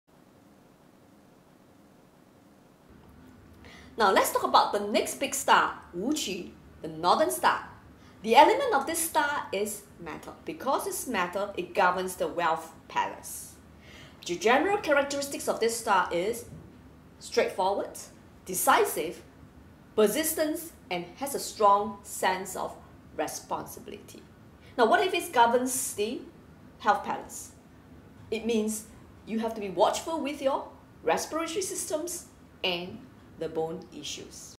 Star of Wu Qu
Zi Wei Dou Shu is a system that has shown me what was fated in my life and what is my predetermined destiny. The information has been useful because it has given me direction so that I could make good choices. The decisions that have made a difference and change my life.  Thank you for your registration to join Zi Wei Dou Shu class commencing 22-23 May 2021.
The short clips on Wu Qu 武曲 explaining the general characteristics may be helpful before you join me.  Wu Qu武曲 is like a warrior that fights, having lots of drives.  Star governs wealth. Various opportunities in generating income when the Star is timely.
When Wu Qu 武曲   appears in Spouse Palace, there is tendency meeting someone who has stronger personality.  Successful WU QU 武曲 cultivates patience.
There are 14 Main Stars, 十四主星；4 Auxillary Stars, 四辅助星 appearing in the 12 Palaces, 十二宫位. To understand the star placement in the palaces, you would need to have your birth year, month, day and hour.